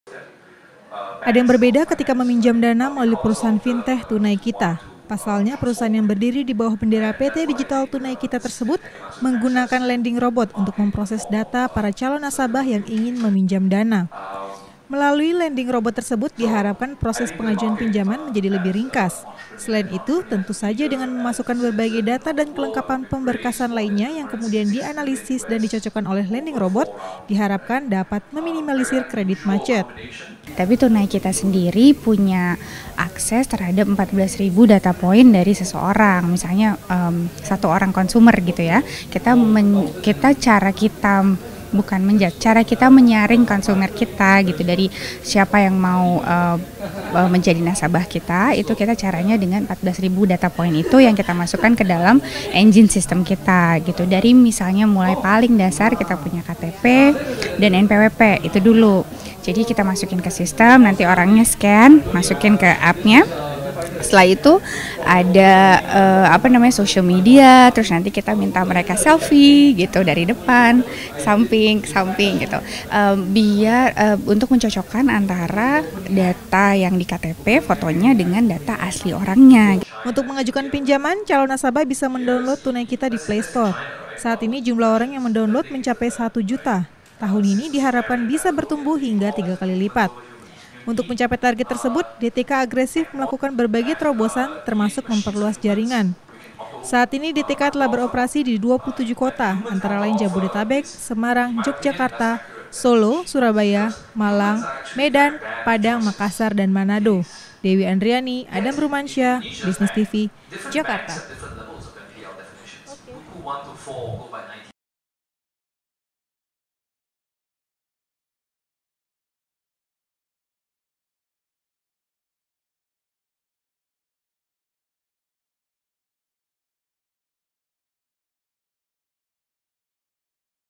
Pakai Lending Robot, Tunai Kita Tekan Kredit Macet
Ada yang berbeda ketika meminjam dana melalui perusahaan fintech Tunai Kita. Ingin tahu bedanya?? Simak video berikut.